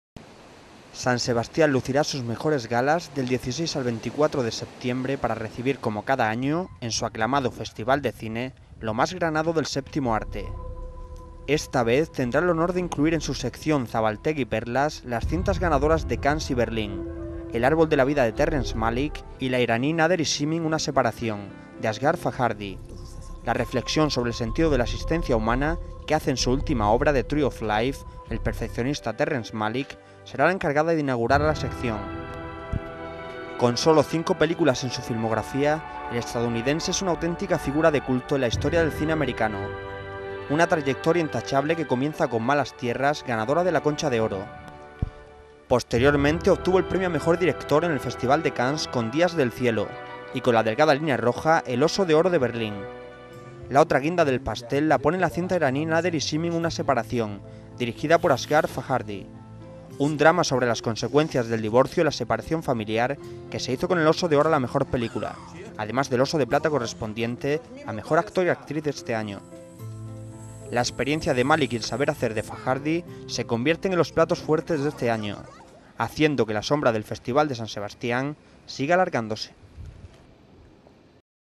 Malick y su 'árbol de la vida' estarán en Donosti
San Sebastián lucirá sus mejores galas del 16 al 24 de septiembre para recibir como cada año, en su aclamado festival de cine, lo más granado del séptimo arte.  Esta vez tendrá el honor de incluir en su sección de Zabaltegi-Perlas las cintas ganadoras de Cannes y Berlín; El árbol de la vida de Terrence Malick, y la iraní, Nader y Simin, una separación; de Asghar Farhadi.